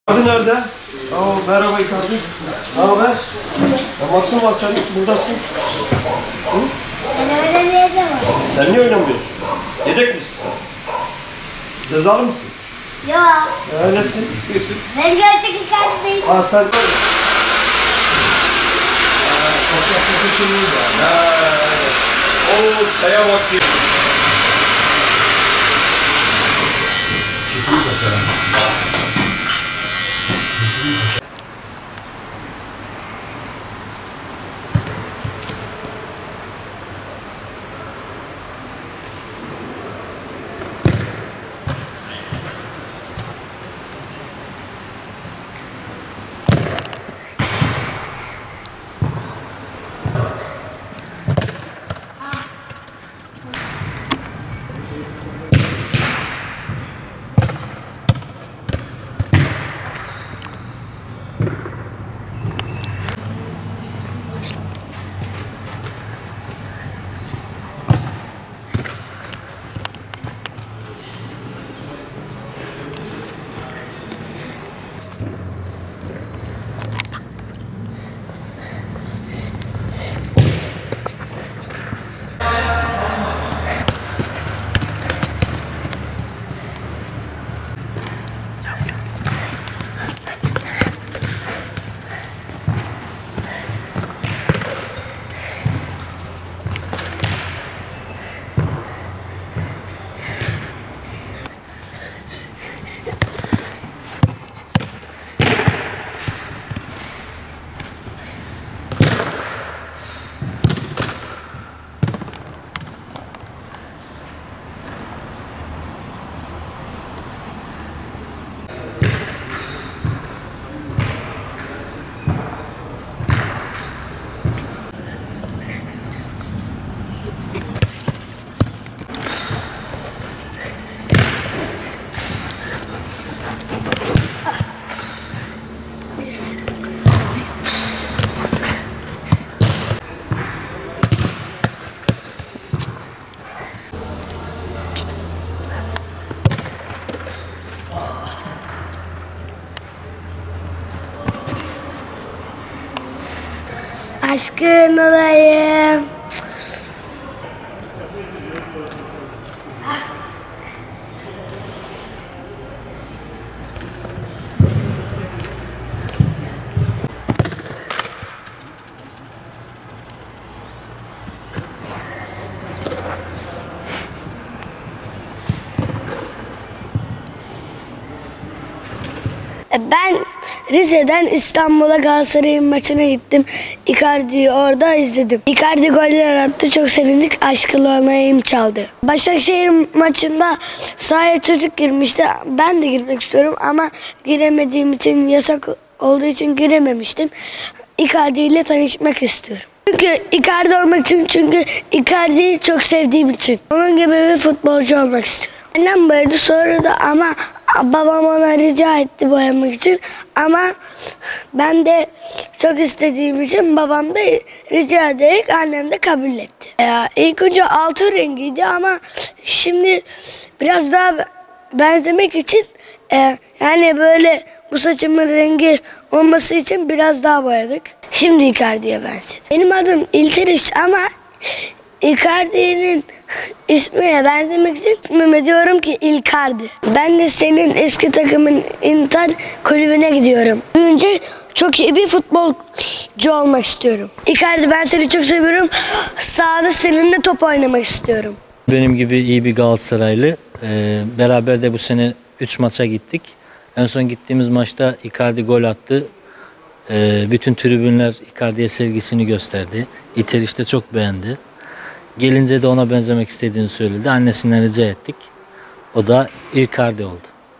İşte Rize’nin Minik İcardi'si İlteriş Memişoğlu
Rize’de yaşayan 6 yaşındaki İlteriş Memişoğlu’nun Icardi sevgisi, saçlarını sarıya boyattırdı.